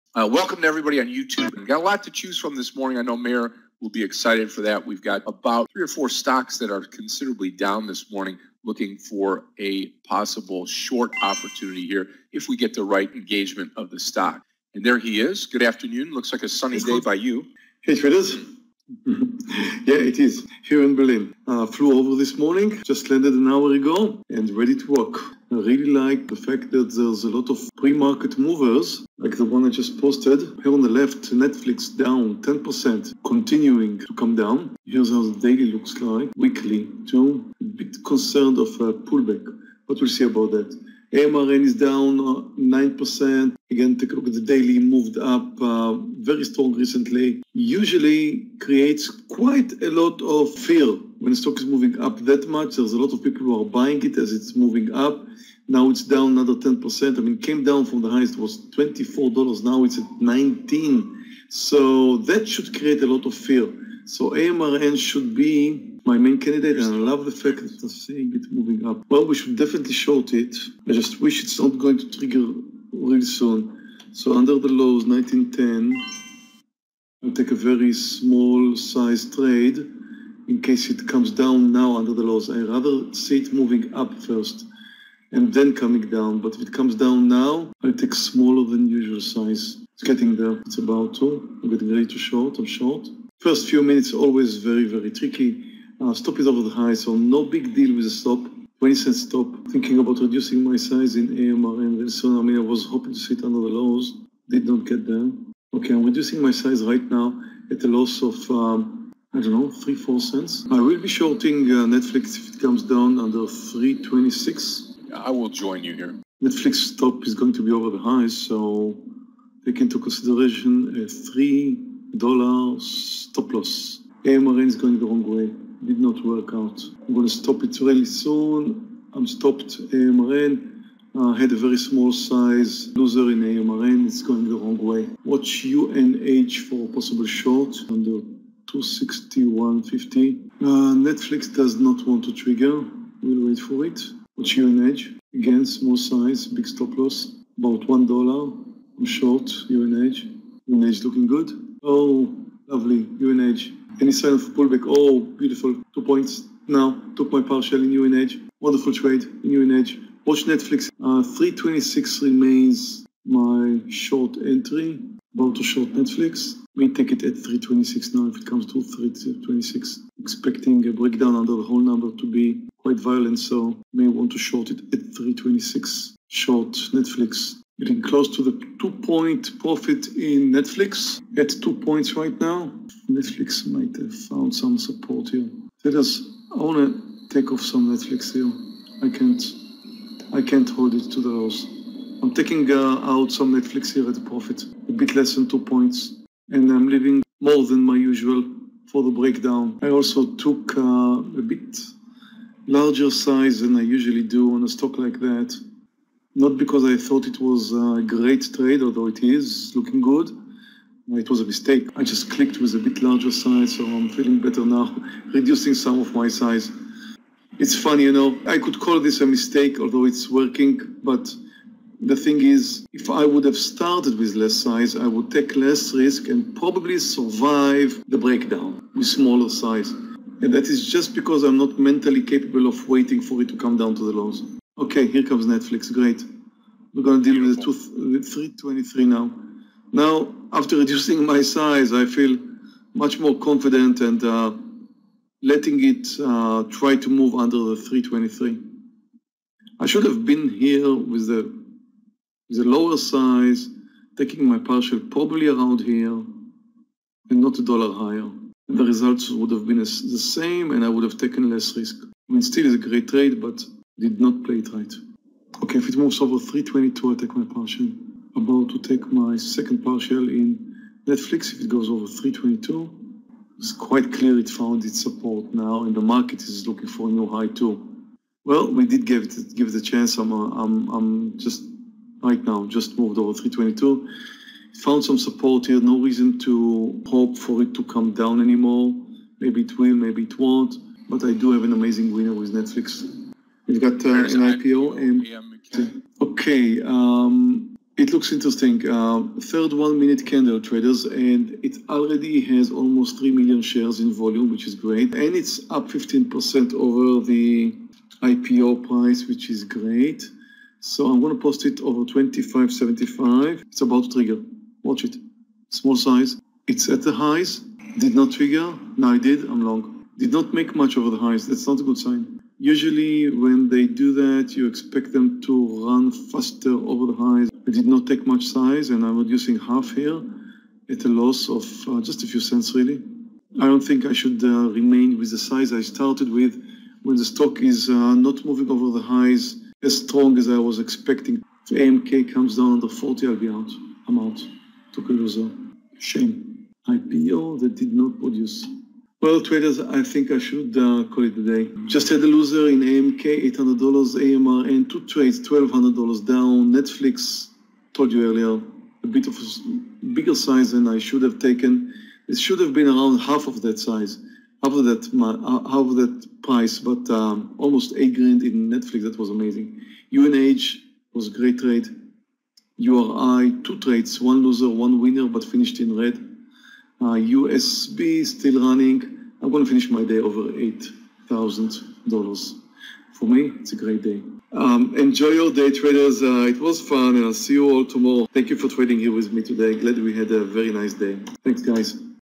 Tradenet Trading Room, July 18: Big Mistake On NFLX! Earned +$8,465.00 In Profits!
Sign up for our trading challenge and win access to a $14,000 funded account